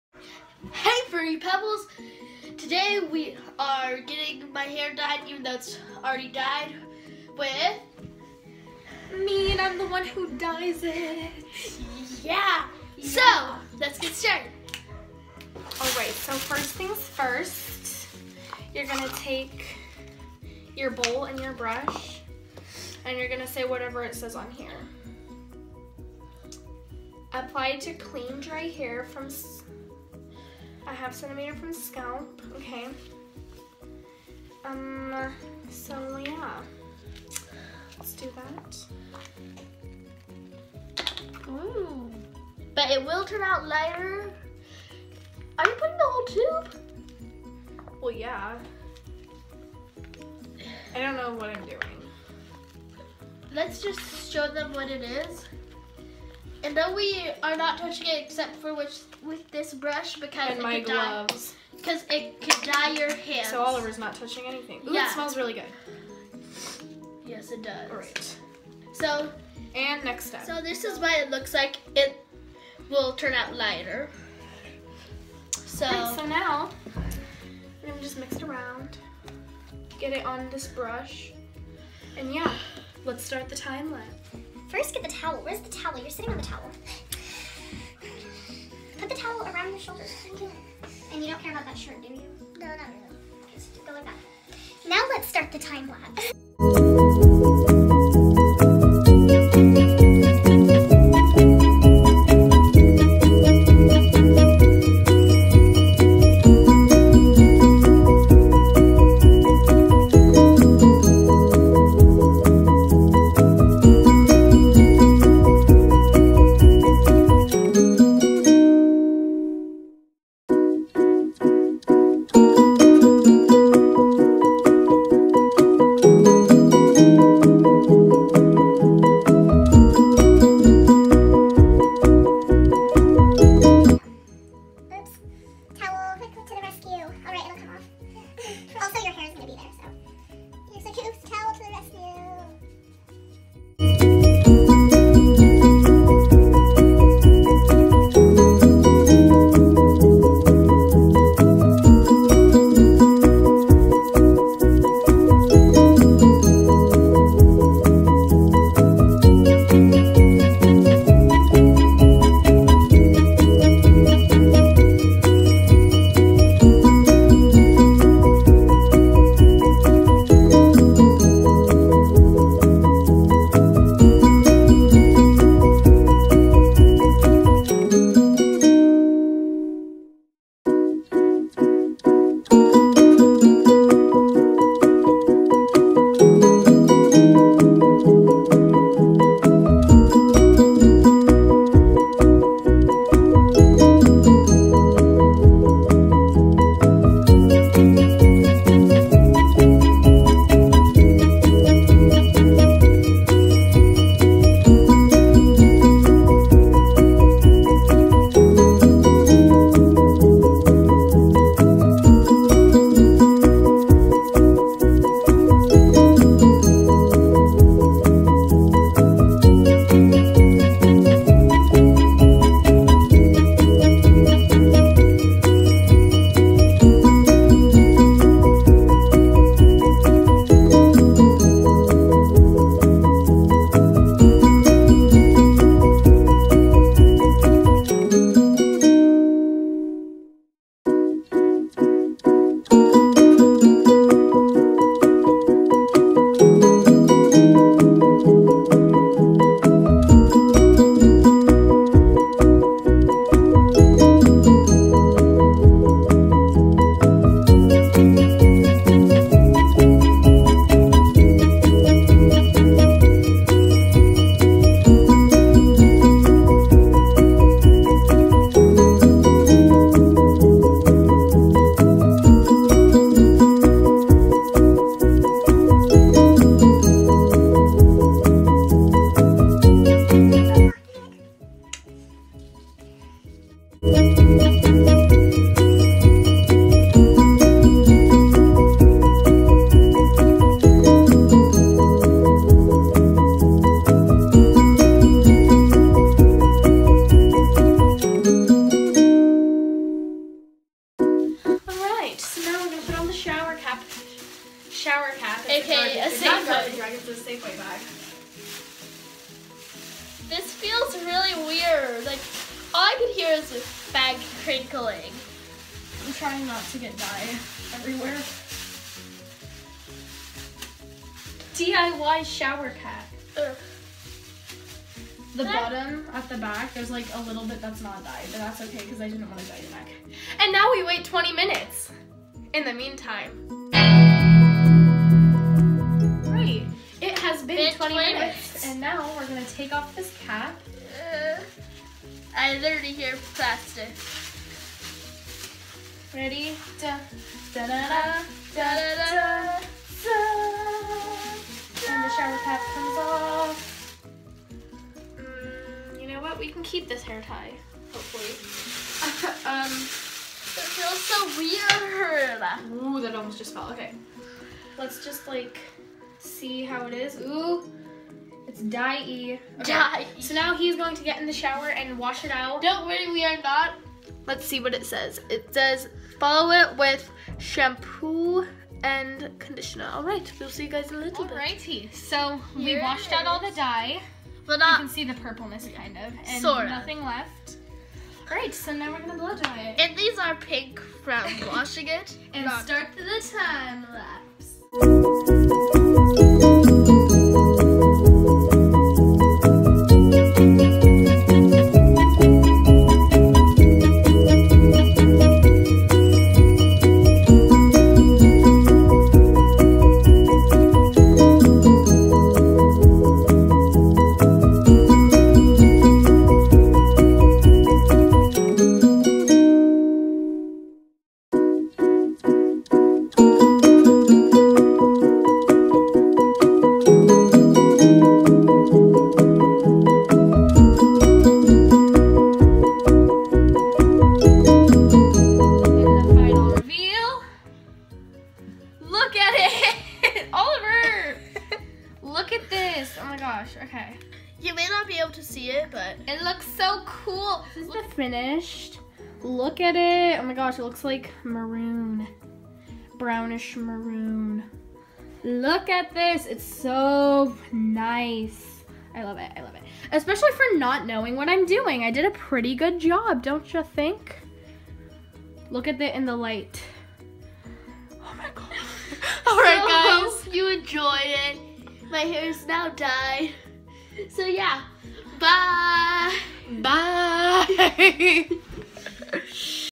My Sister Dyes my Hair Purple
Today my sister dyes my hair from home.